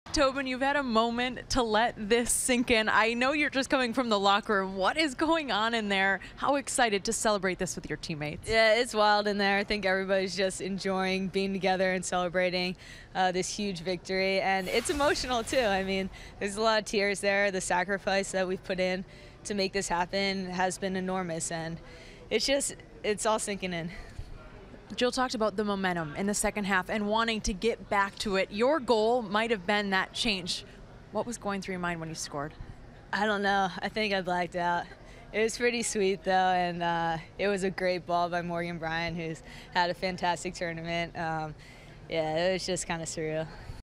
Tobin Heath describes locker room celebration after World Cup win
Tobin Heath talks about the celebration in the USWNT locker room after winning the 2015 FIFA Women's World Cup.